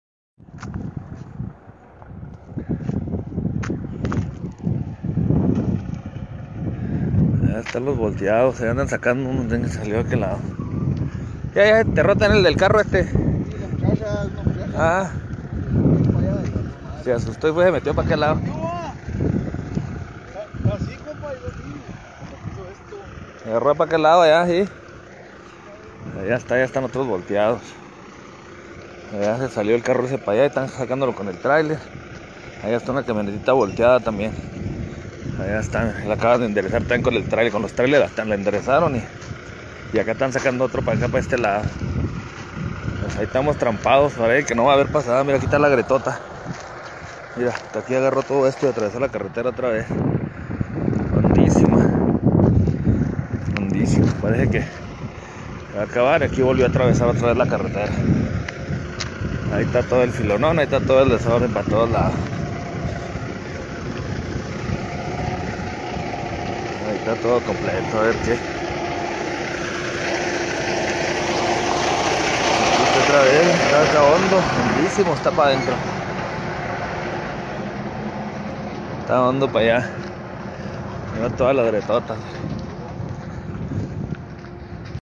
Se abre la tierra ahora en Chihuahua.
Se abre la tierra en Chihuahua.
Mega grieta en la carretera Camargo-Ojinaga, autoridades bloquearon el paso a los automovilistas, para evitar ponerlos en riesgo.